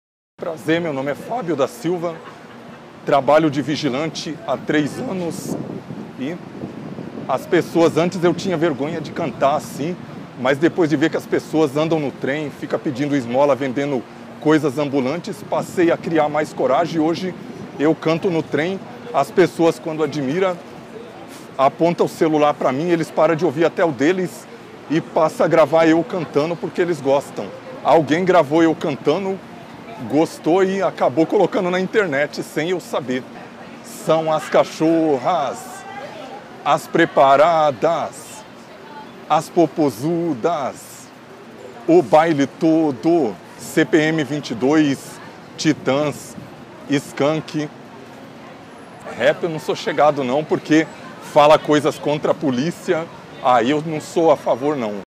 Cantor do Metro Fábio da Silva Part 1
Homem cantando no metrô e na cptm vira febre na internet.

ஜ۩۞۩ஜ▭▭▭▭▭▭▭▭▭▭▭▭▭ LEIA A DESCRIÇÃO DO VÍDEO ▭▭▭▭▭▭▭▭▭▭▭ஜ۩۞۩ஜ▭▭▭▭▭▭▭▭▭▭▭▭▭ ☆SE GOSTOU DO VÍDEO .